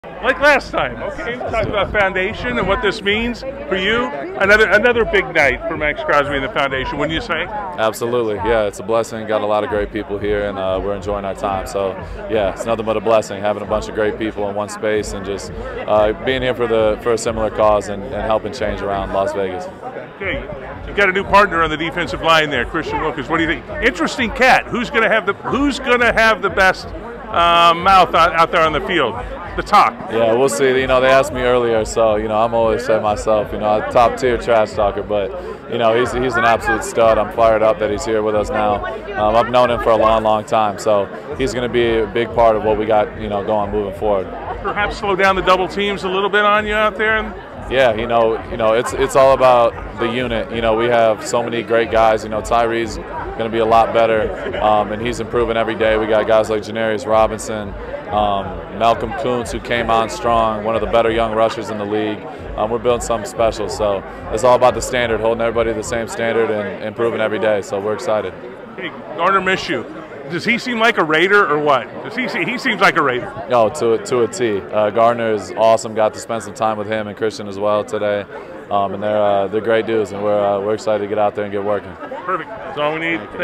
Raiders DL Maxx Crosby on arrival of new players to the team. March 14, 2024
Las Vegas Raiders defensive lineman (edge rusher) Maxx Crosby talks to Ron Futrell about his Foundation and the arrival of new players Christian Wilkins and Gardner Minshew. March 14, 2024.